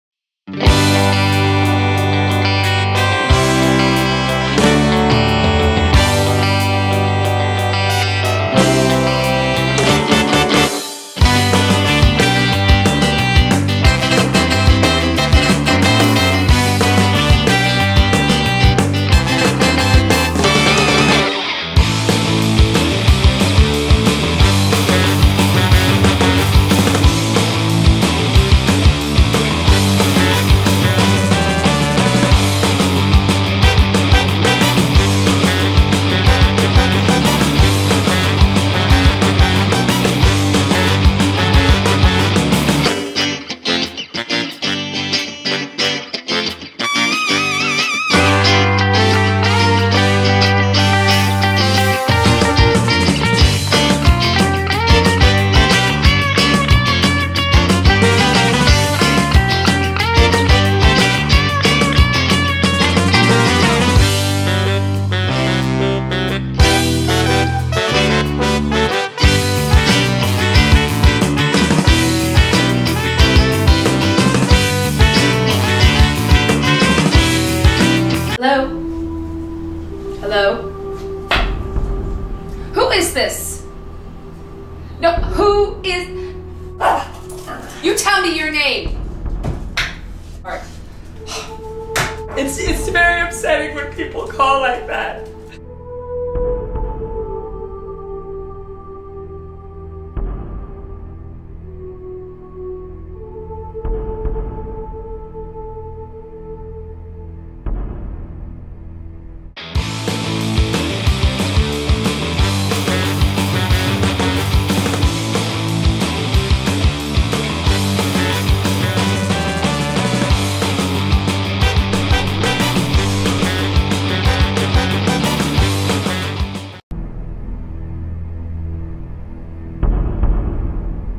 Bowling Day
(8) Frankie and Geri go bowling. There is another unwanted phone call.